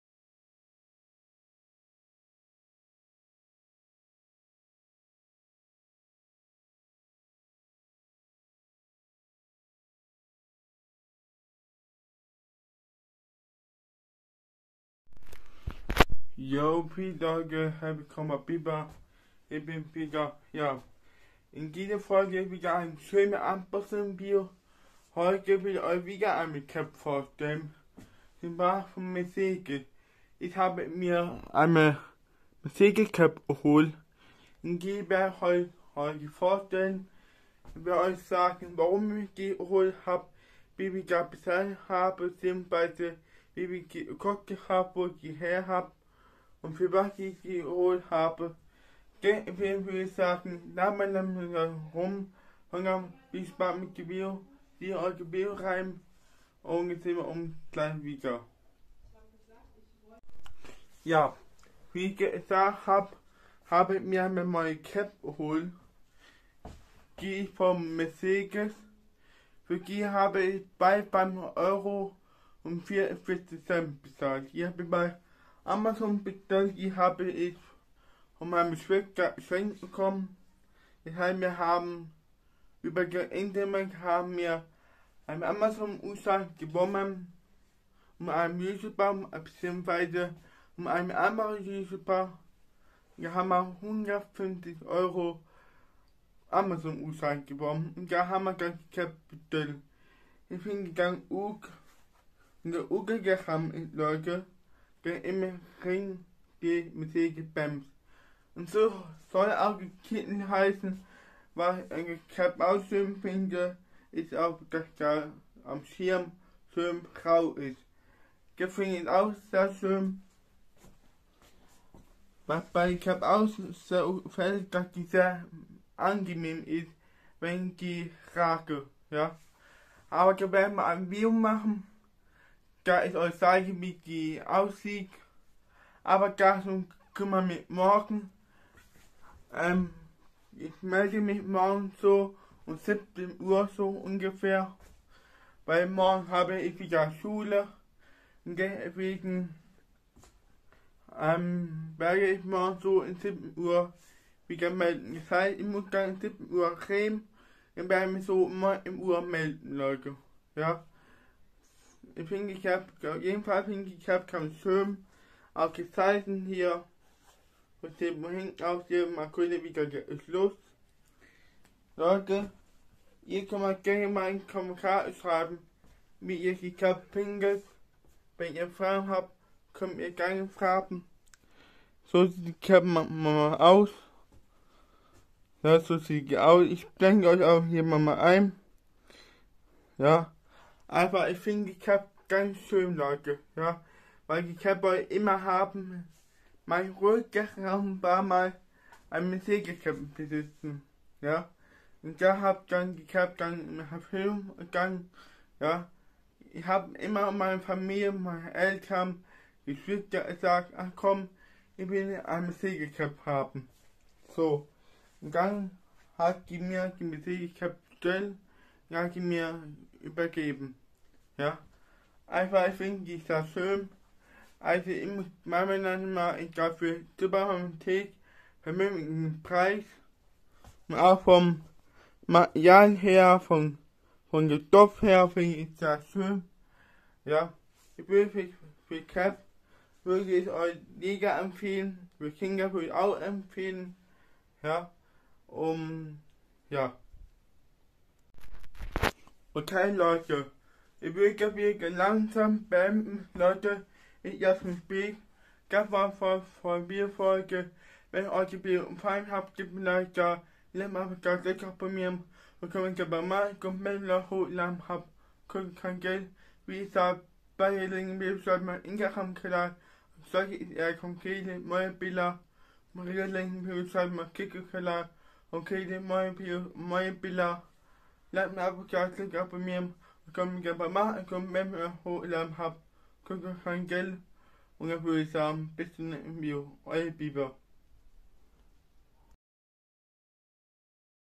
Die Cap von Mercedes-Benz (Unboxing Video) !!|Bieber
ich hoffe euch hat das Video gefallen und konnte euch einen schönen Einblick zeigen

Name : Justin
Alter : 18 Jahre alt
Beruf:Youtuber und Model
Wohnort:  Hockenheim baden Württemberg
Größe: 180 cm

LG Fortsetzung Folgt eure Bieber ✌🏻